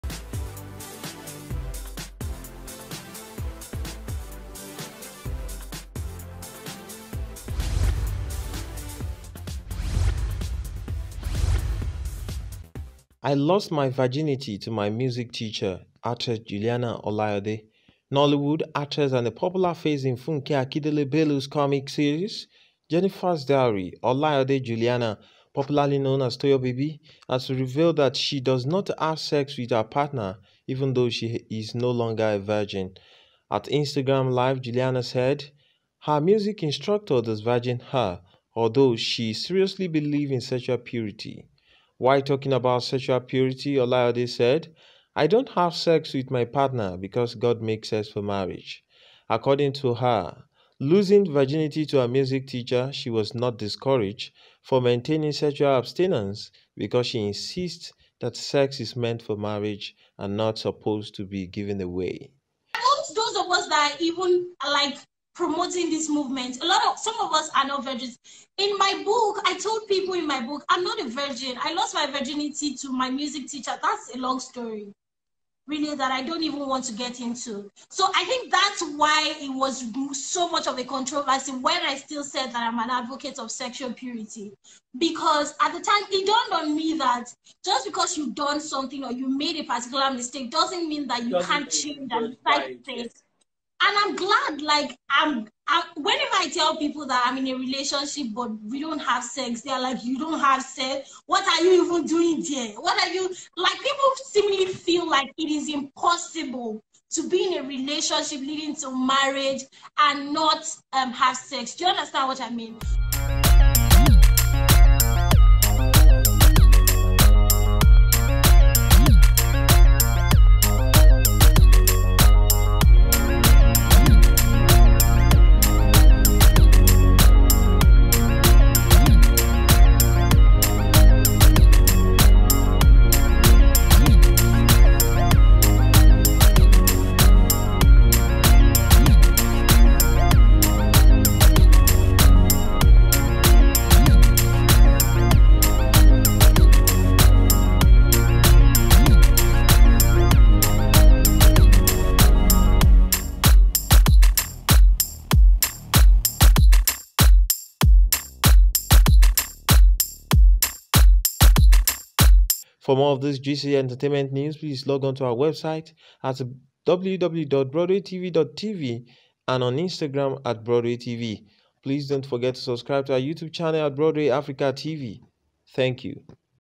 “I Lost My Virginity To My Music Teacher" - Actress Juliana Olayode
Nollywood actress and a popular face in Funke Akindele-Bello’s comic series, ‘Jenifa’s Diaries’, Olayode Juliana, popularly known as Toyo Baby has revealed that she does not have sex with her partner even though she is no longer a virgin.